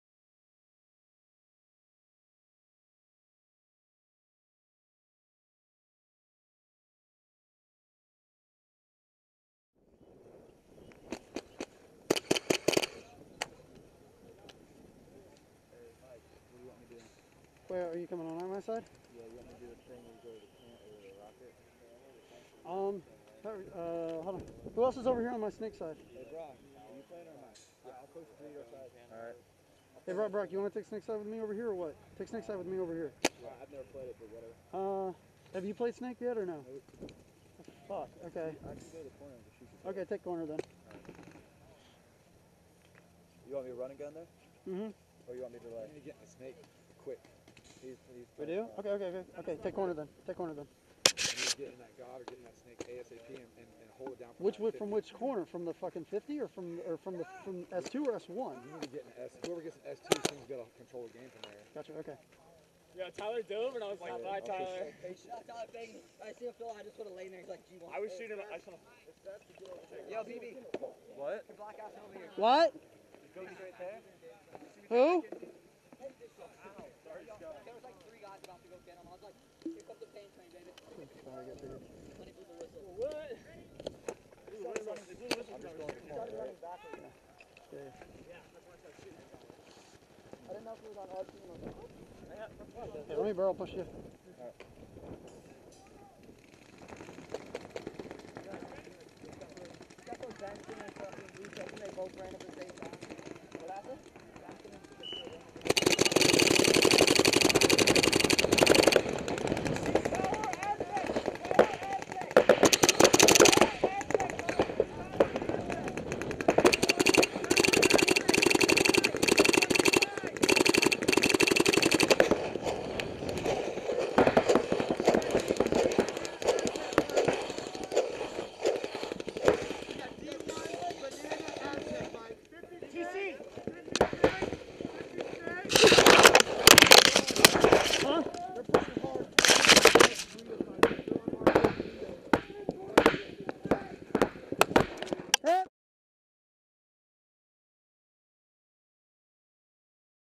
Autococker SR testing - XBall
Testing out the Autococker SR, just testing it out for an upcoming review!

Sorry about the camera falling off the mask, I hit it with the loader while crawling in the snake.